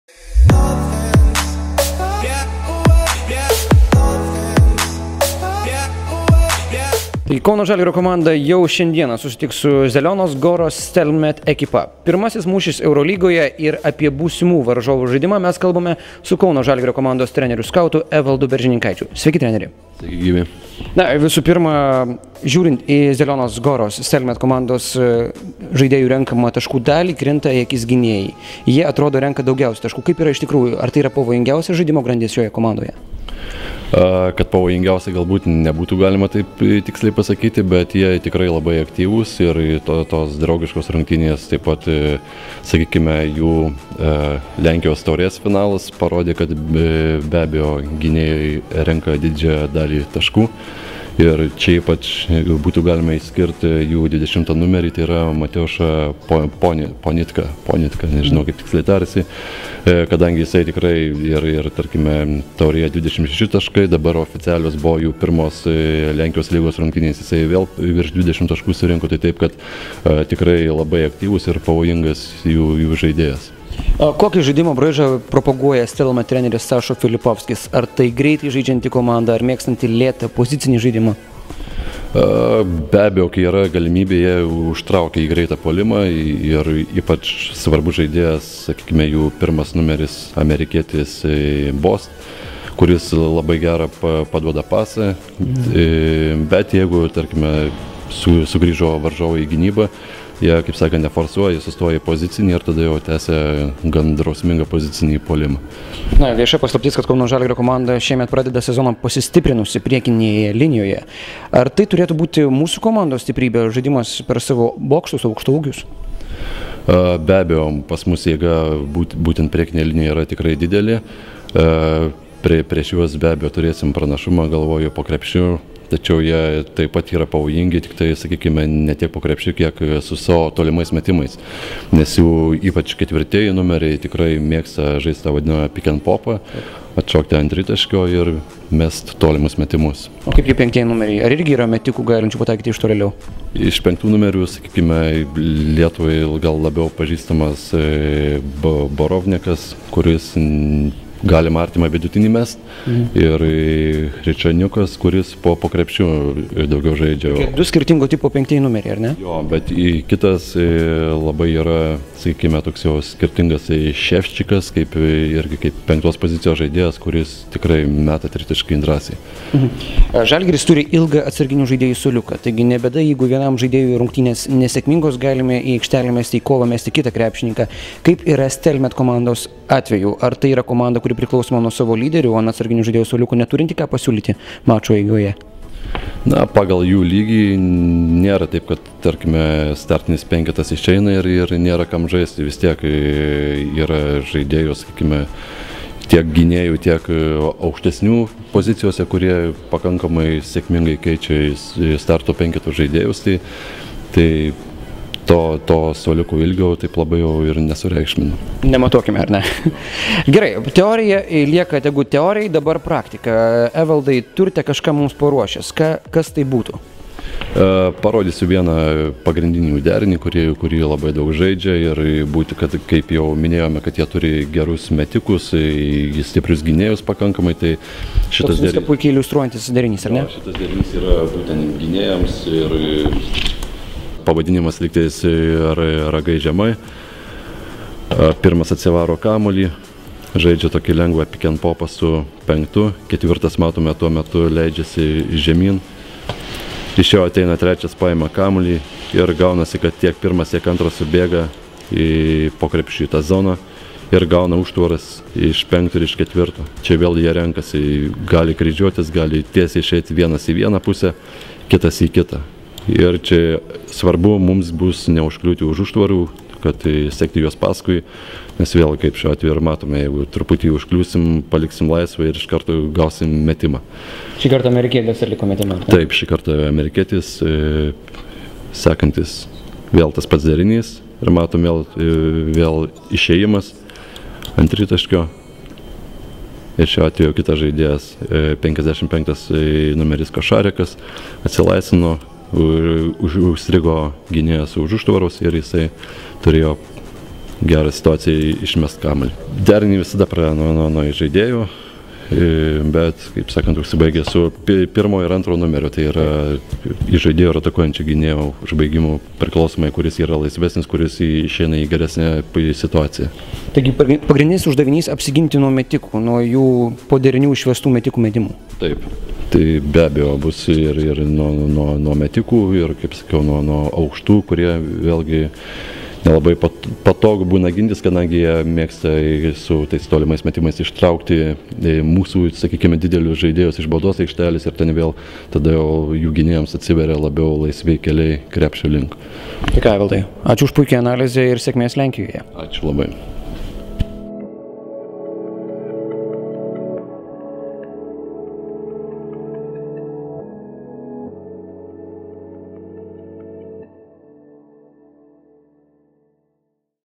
DėmesysEurolygai. „Stelmet“ žaidimo analizė
Kauno „Žalgirio“ krepšininkai jau šį vakarą pradės kovas naujojo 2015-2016 m. sezono Eurolygos turnyre. Žalgiriečių varžovais Lenkijoje bus šalies čempionė Zielona Guros „Stelmet“ ekipa.

Į Eurolygą po vienerių metų pertraukos sugrįžtantis slovėno Sašo Filipovskio treniruojamas klubas ekspertų dažniausiai neminimas tarp kandidatų patekti į „Top 16“ etapą, tačiau galima neabejoti – namuose savų žiūrovų palaikoma „Stelmet“ bus itin pavojingas oponentas visoms C grupės ekipoms.

Kalbėdamas „Žalgiris TV“ laidoje DėmesysEurolygai kauniečių komandos treneris-skautas Evaldas Beržininkaitis teigė, jog Zielona Guros klube išsiskiria mažesnių gynėjų grandis.